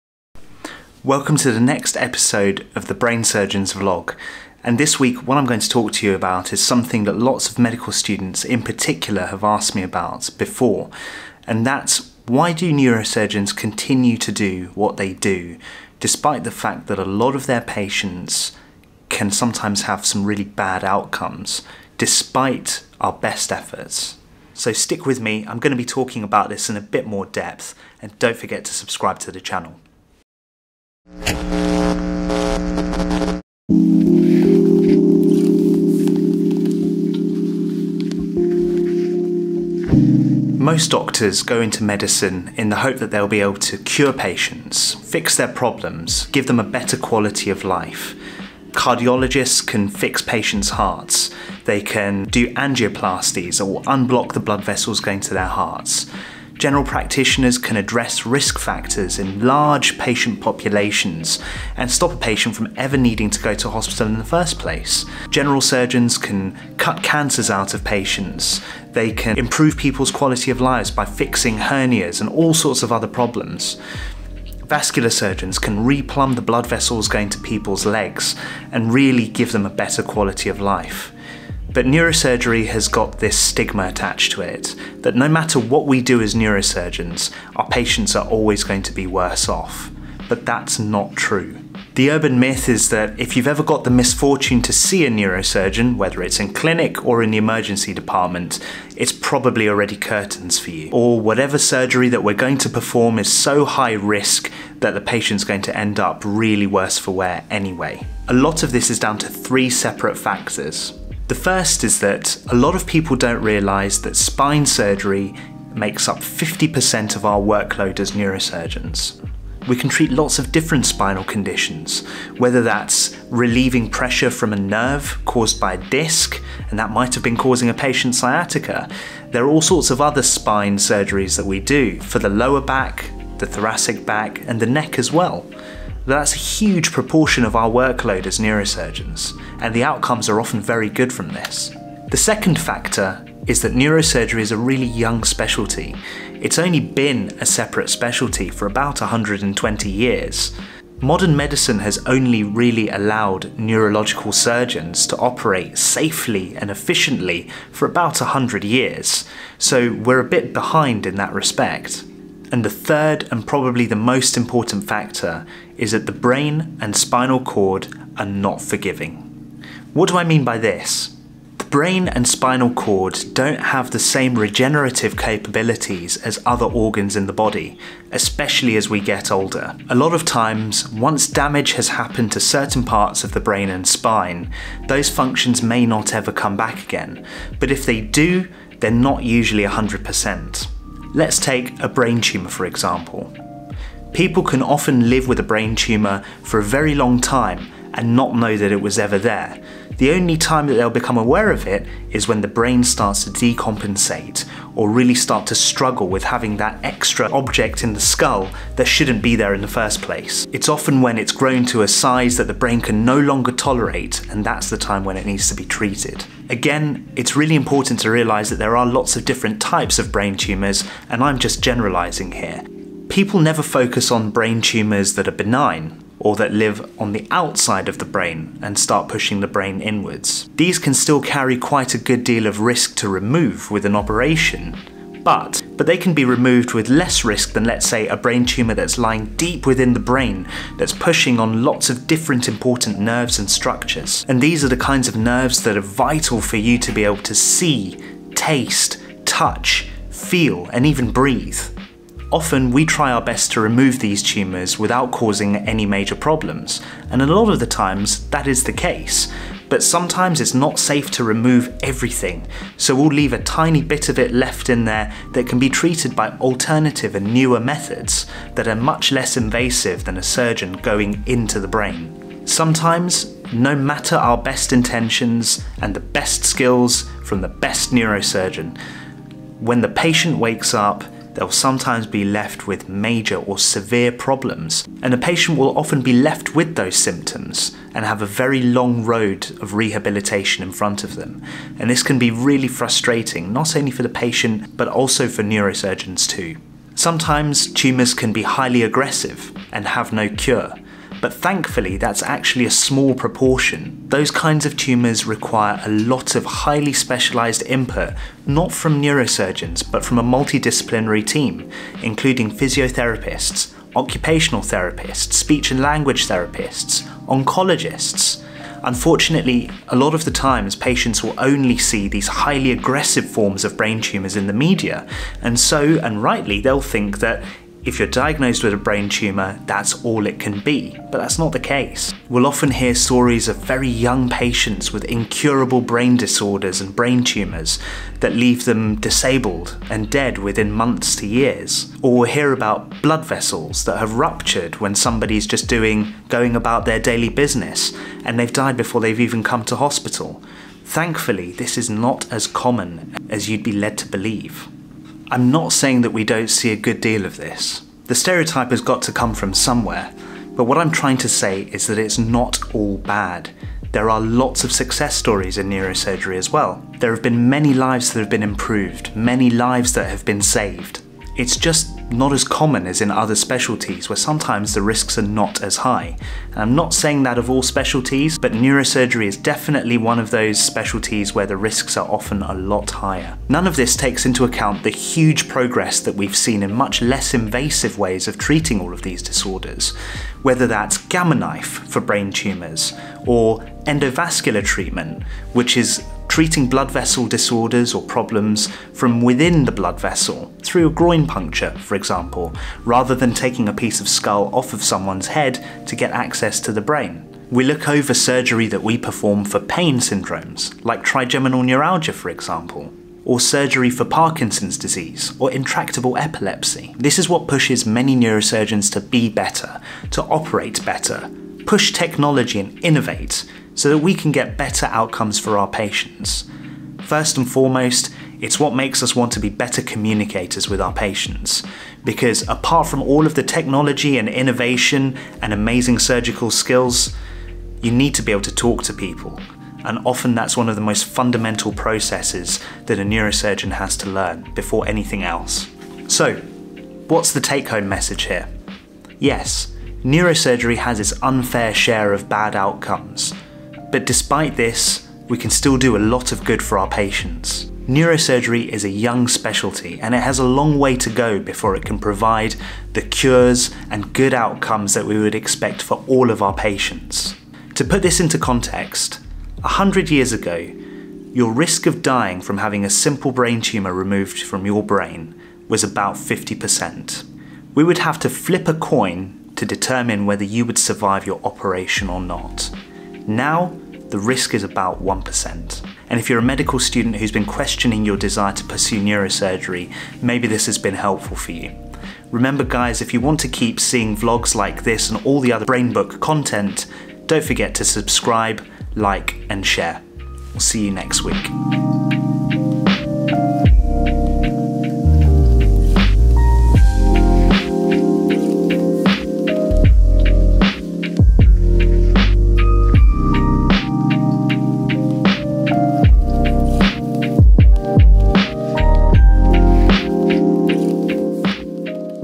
The Brain Surgeon's Vlog - ep. 8 - Patient outcomes in Neurosurgery
I often get asked about patient outcomes in neurosurgery. Is it true that we're always breaking bad news? Do all of our patients do really badly? If that's the case, why do we bother doing what we do?

A lot of medical students have an interest in becoming neurosurgeons but they can be put off by their limited exposure and seeing lots of bad outcomes in our patients, most often after a brain injury.

In this vlog I'm going to explore some of these common misconceptions about neurosurgery!

Thanks to Brandon for the awesome thumbnail!